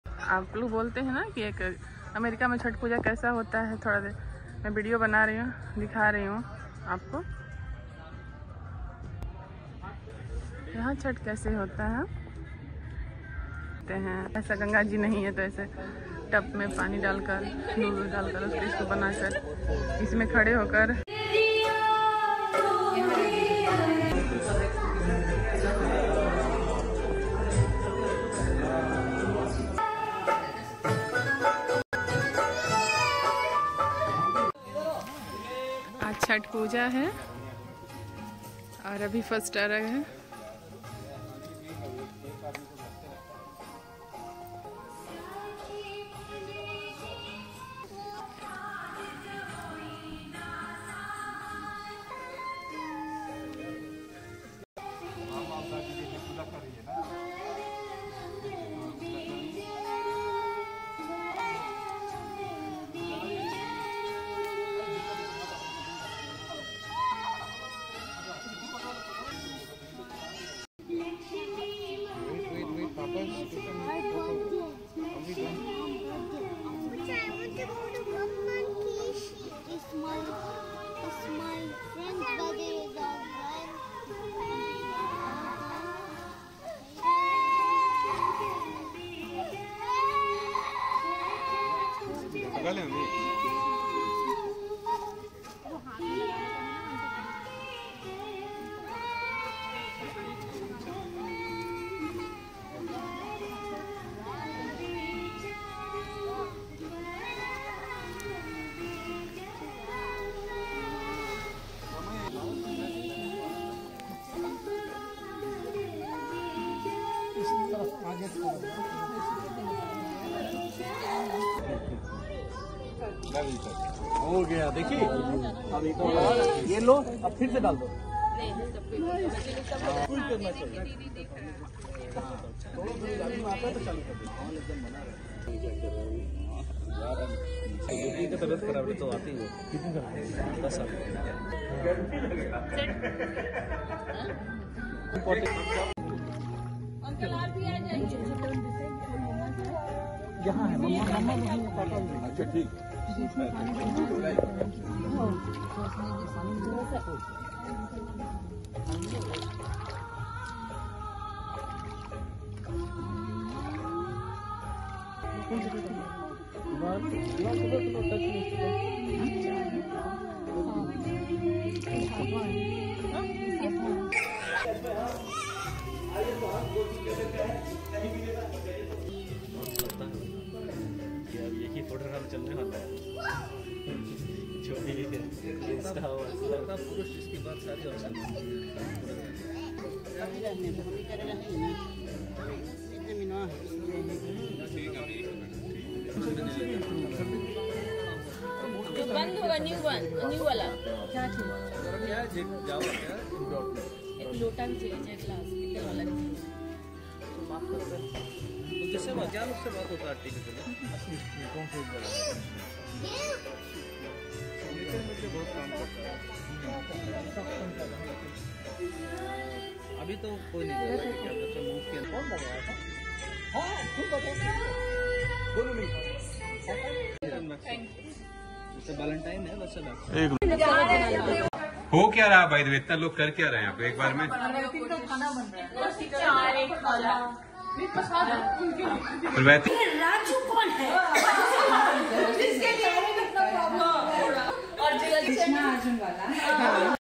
Indian Americans | Celebrate Chhath Puja in The USA | Jai Ho chhathi maiya
** Indian Americans in the USA celebrated Chhath Puja, a festival that honours the Sun God and nature. The festivity includes prayers, rituals, and cultural heritage sharing with vibrant processions and folk songs, showcasing the lively spirit and warmth of the celebrations. **